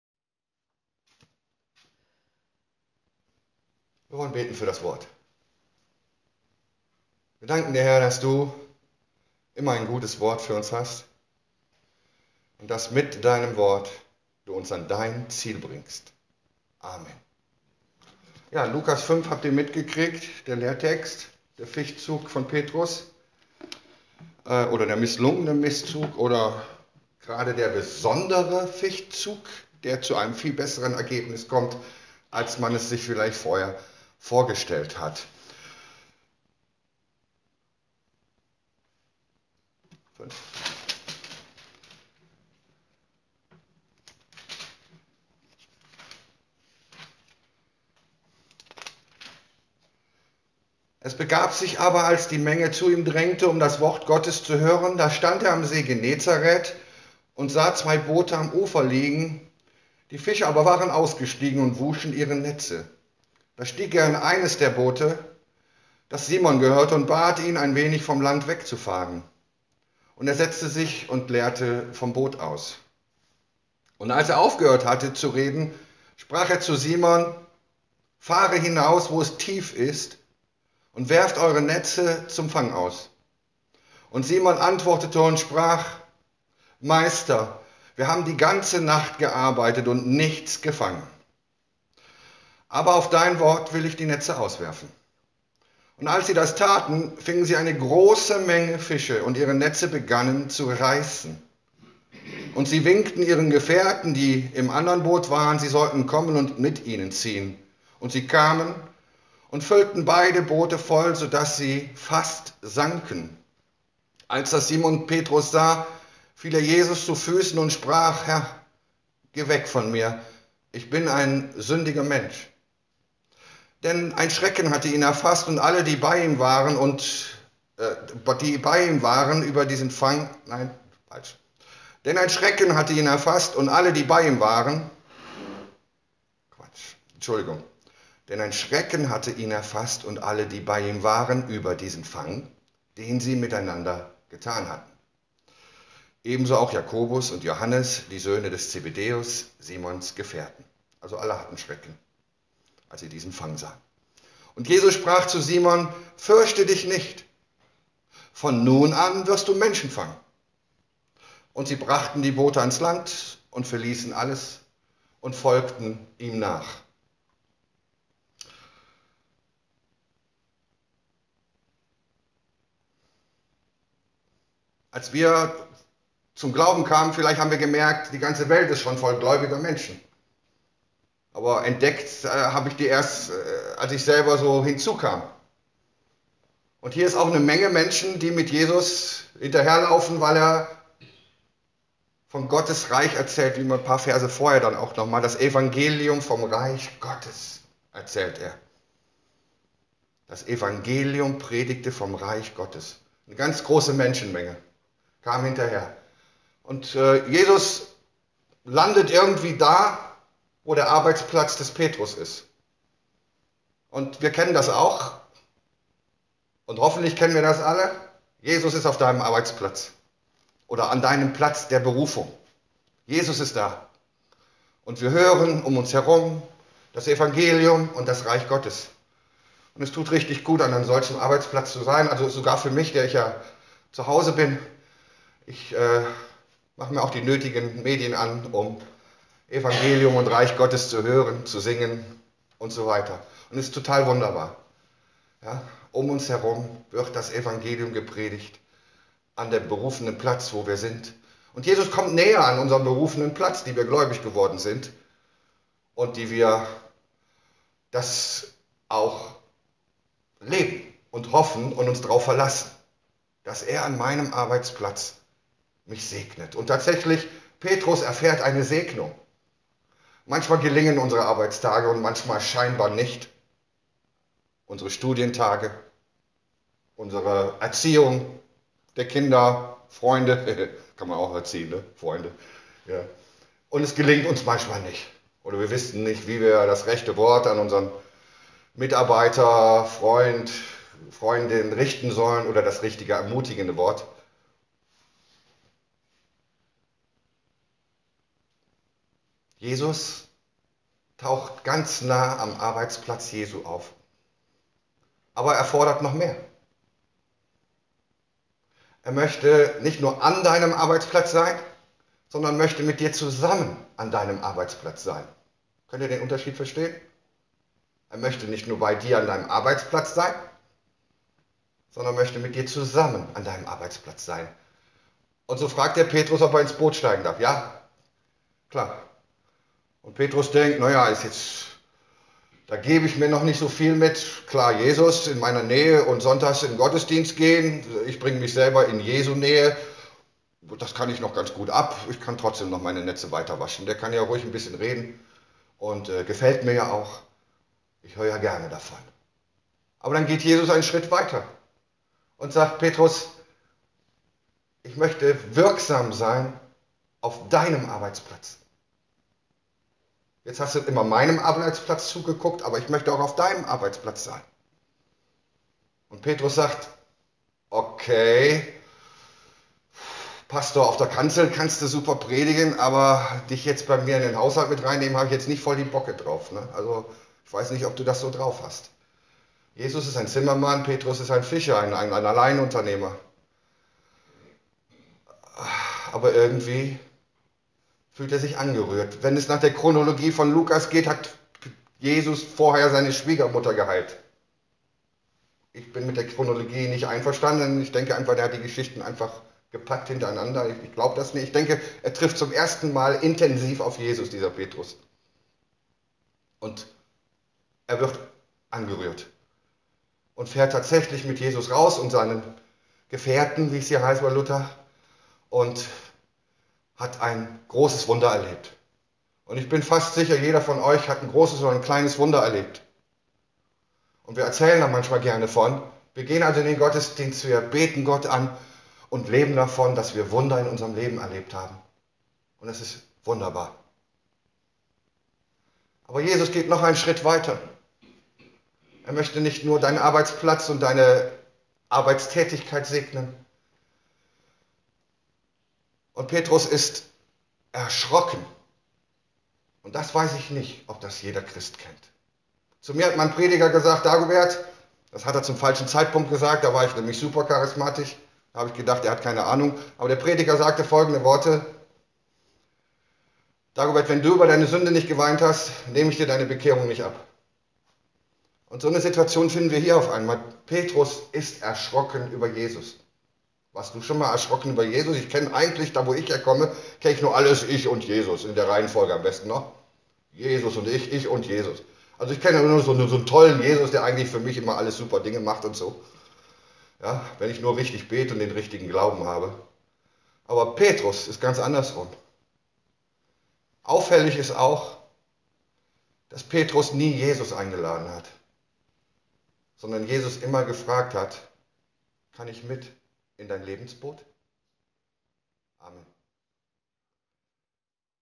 Schrecklicher Gott!
Es ist eher typisch für uns, dass wir vor Freude in die Hände klatschen, als dass wir vor Scham oder Tränen rot werden